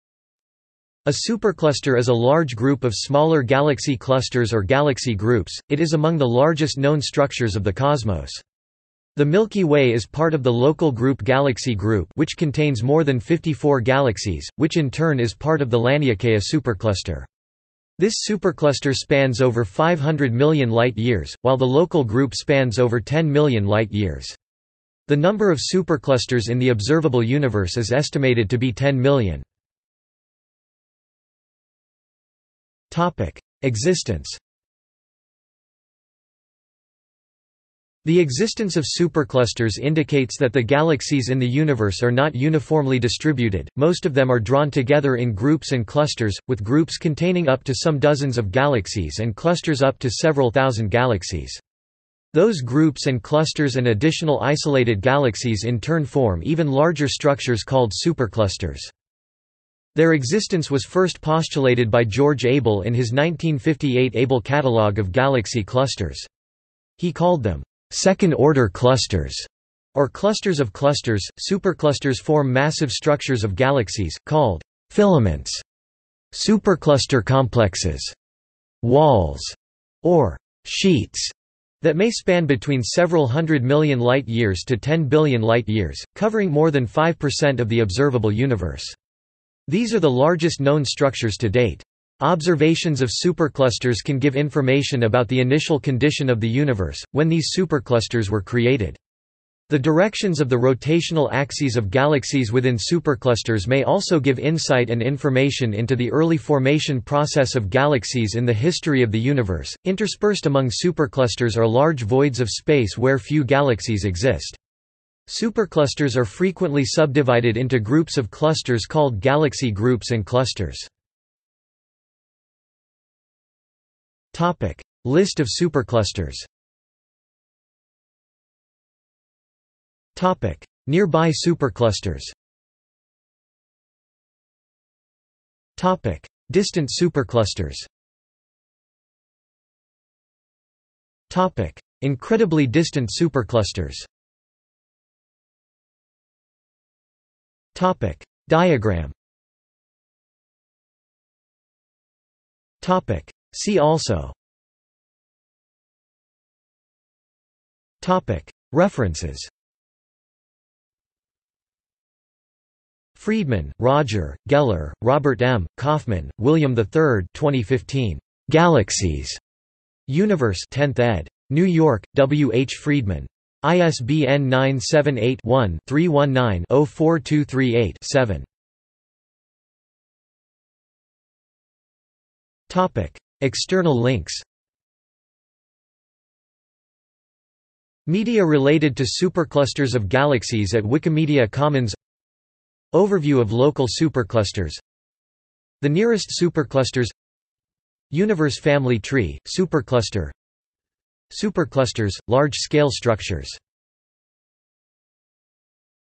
Supercluster | Wikipedia audio article
- Socrates

SUMMARY
A supercluster is a large group of smaller galaxy clusters or galaxy groups; it is among the largest-known structures of the cosmos. The Milky Way is part of the Local Group galaxy group (which contains more than 54 galaxies), which in turn is part of the Laniakea Supercluster. This supercluster spans over 500 million light-years, while the Local Group spans over 10 million light-years. The number of superclusters in the observable universe is estimated to be 10 million.